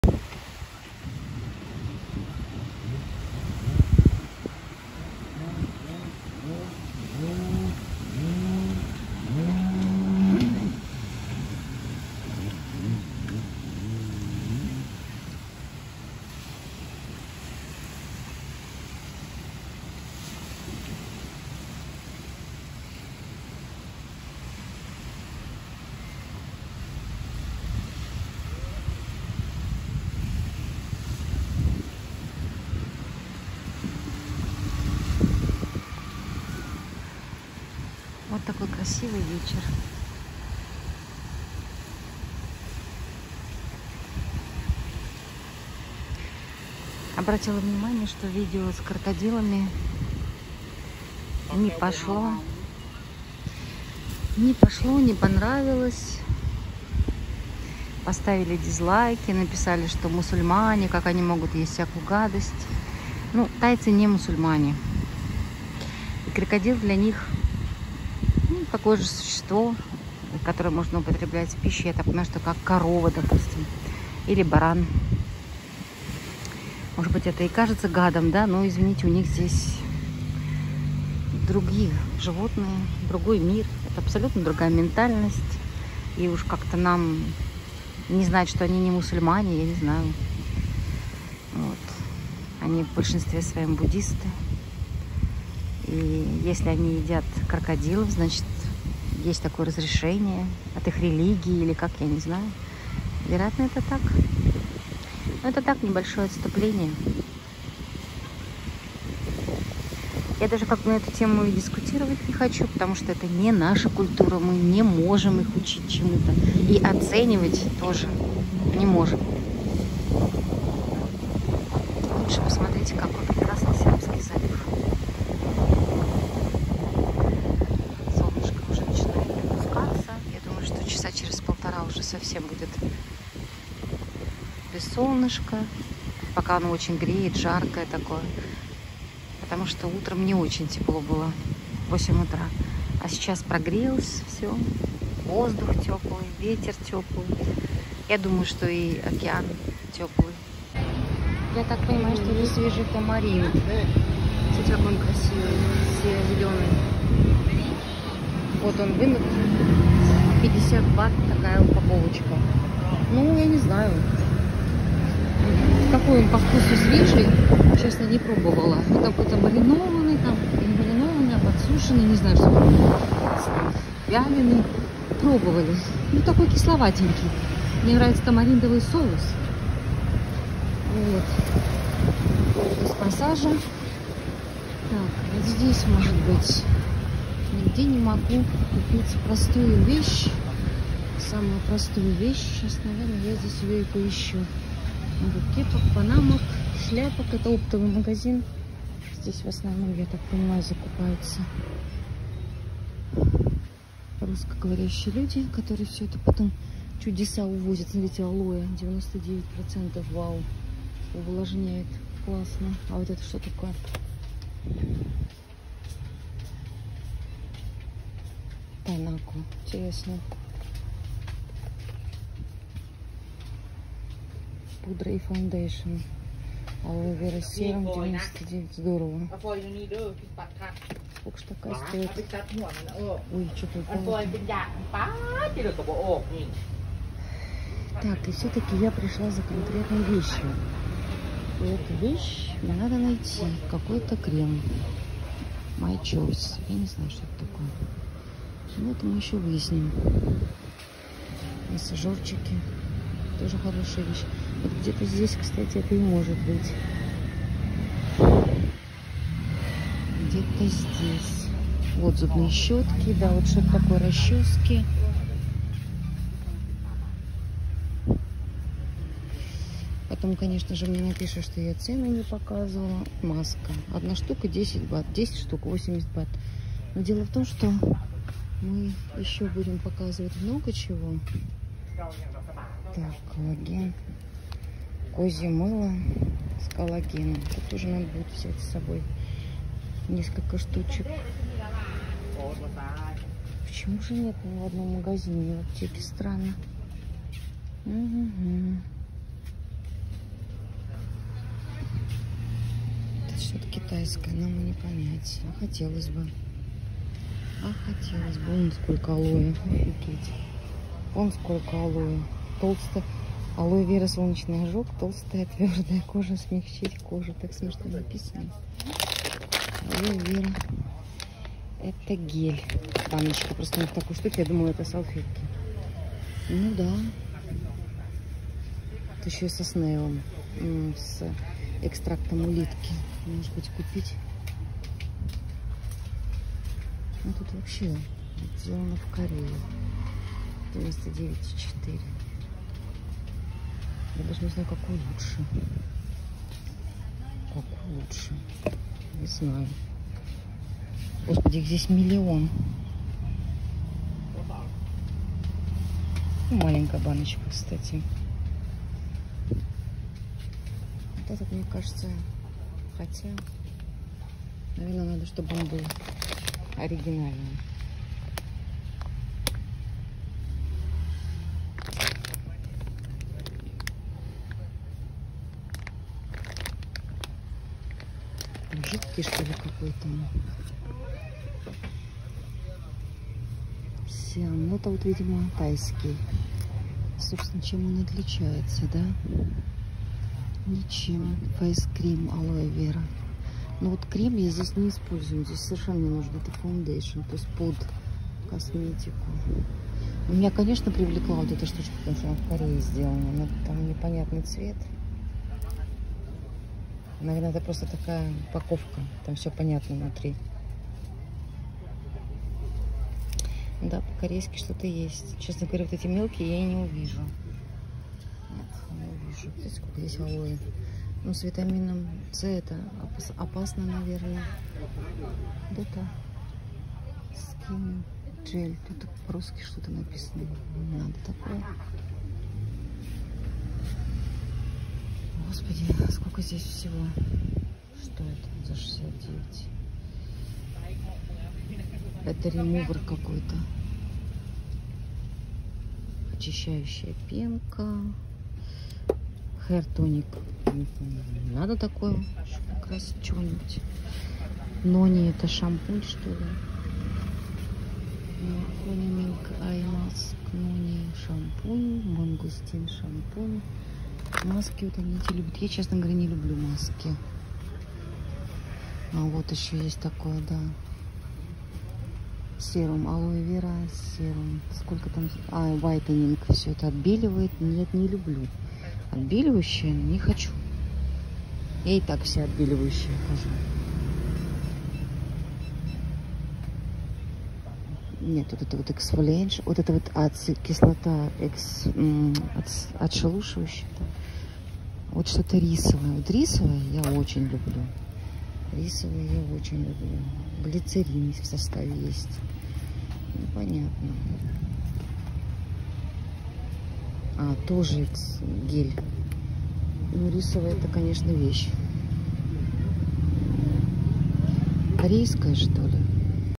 5. Thailand, 2023. Какое разное Алое Вера в Тайланде! Прекрасный закат на Джомтьен.
Я рада приветствовать всех на моем канале!!!

Также , можно вступить в мою группу  на канале Телеграмм

Я в Тик-Ток